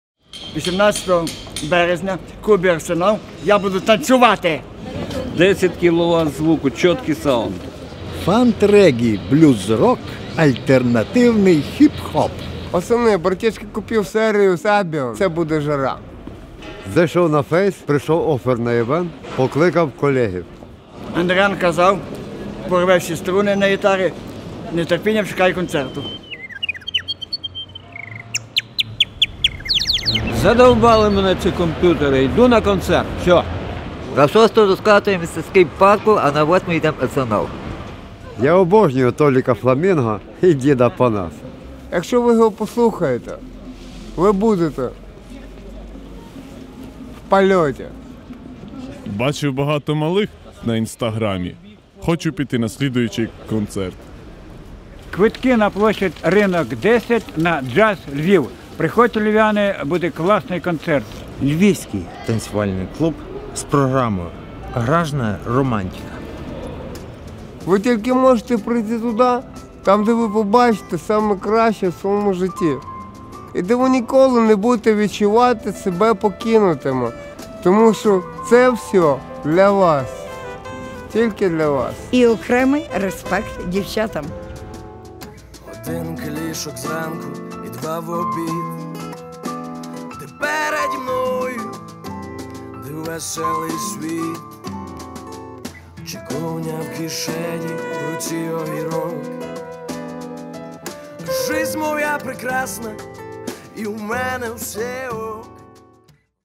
Найдушевніше відеозапрошення на концерт Lvivdanceclub
18-го березня 2016 - Великий Сольний Концерт гурту Lvivdanceclub (при участі Dj Shon (KICKIT)).

Прийди на перший концерт у оновленому складі. На тебе чекає велика програма з нових та старих пісень.

Квитки:
💰 70 грн передпродаж* (на вході 100 грн)
📌 Туристичний центр JustLvivIt, Площа Ринок, 10

Звук - Ткачук Андрій